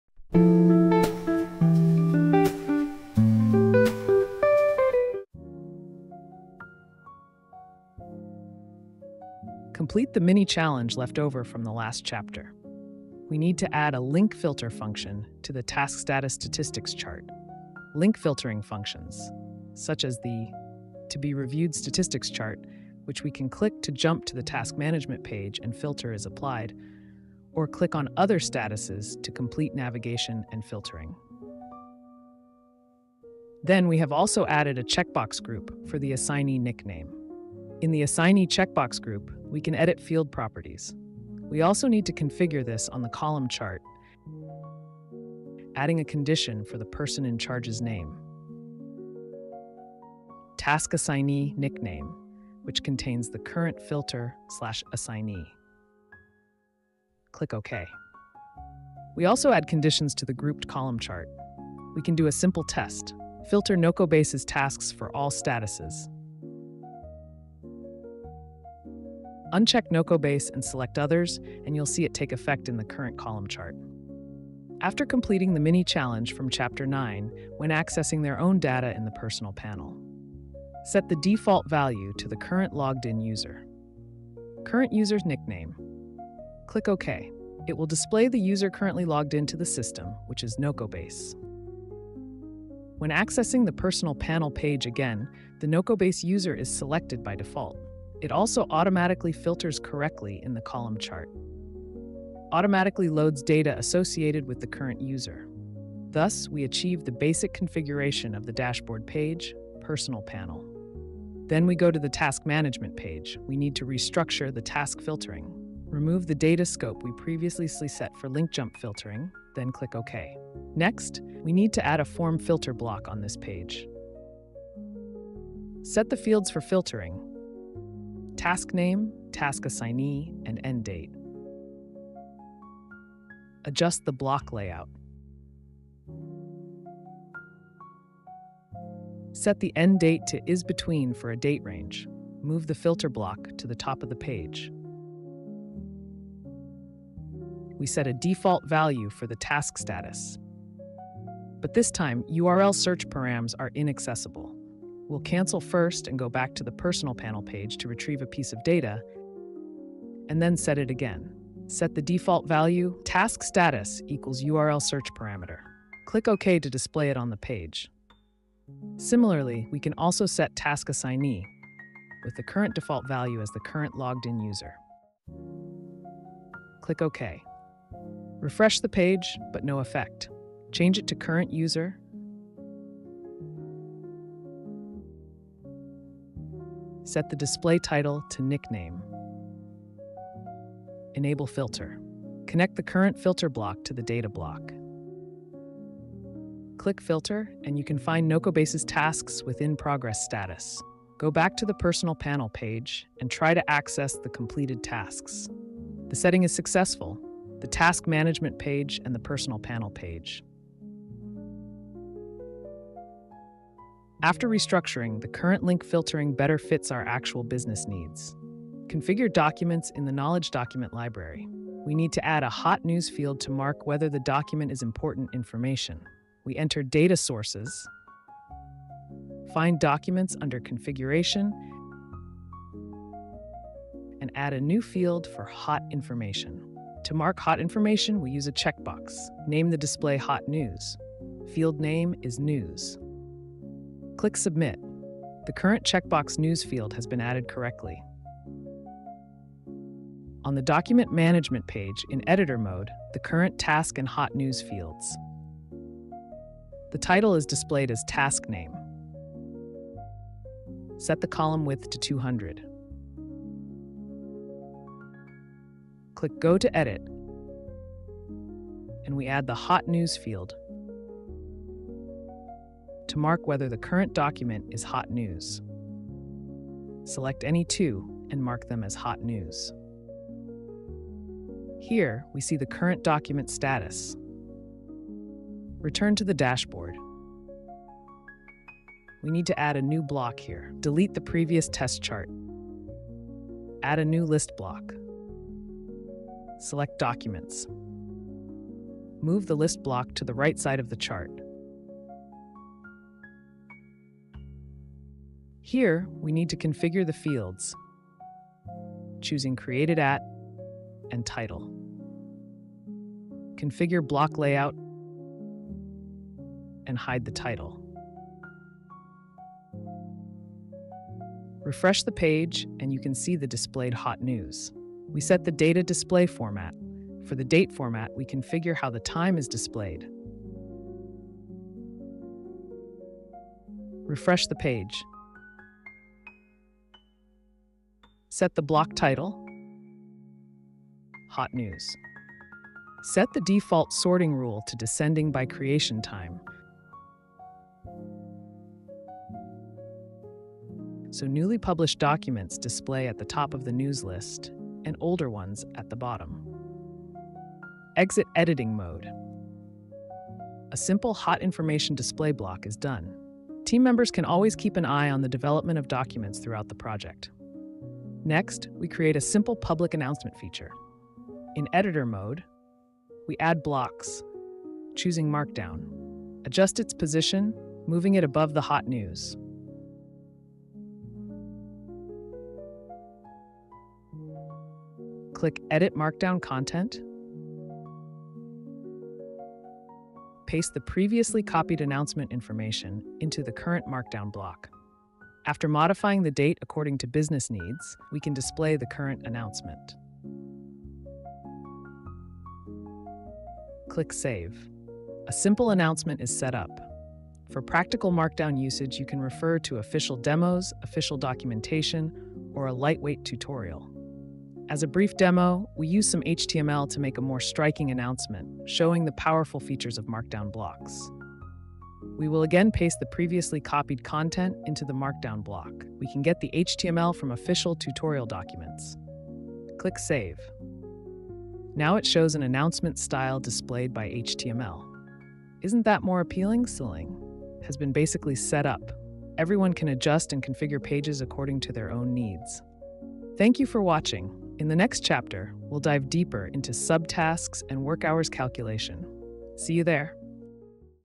NocoBase 101: Task Dashboard (Part 2) - Filter & Conditions-10
In this chapter, we will guide you step by step through the next part of creating a task dashboard. If you have any questions, feel free to consult the forums at any time.

Let’s start this exploration journey by reviewing the content from the previous chapter!

This is Chapter 10 of our tutorial series: Task Dashboard (Part 2) - Filter & Conditions.